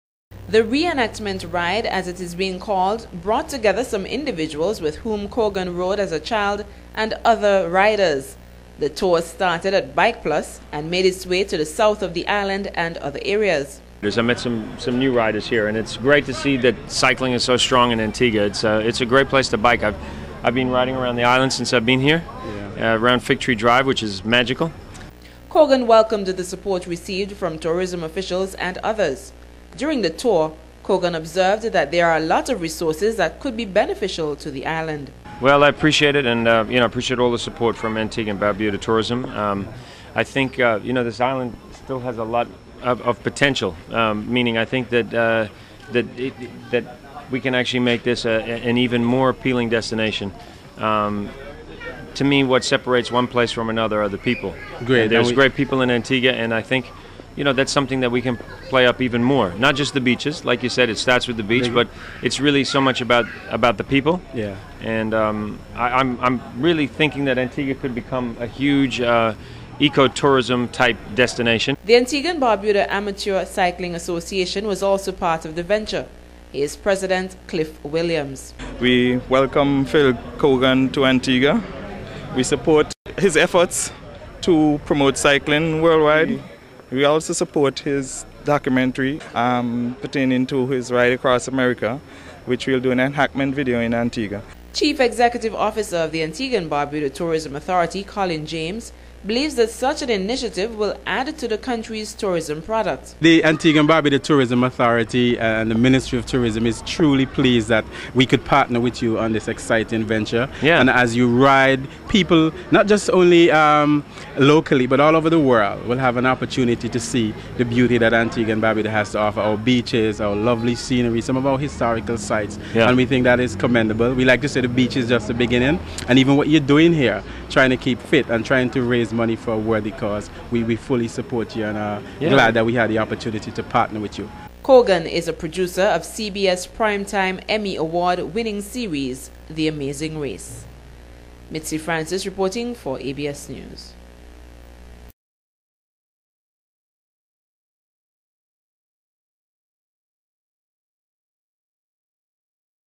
Phil Keoghan Reunion Ride - Sunday 24th Jan 2010 - Antigua
Phil Keoghan lived in Antigua and Barbuda some 30 years ago where he started cycling with island's elite riders during that time, now he return to have a ride with the elite and leisure clubs in 2010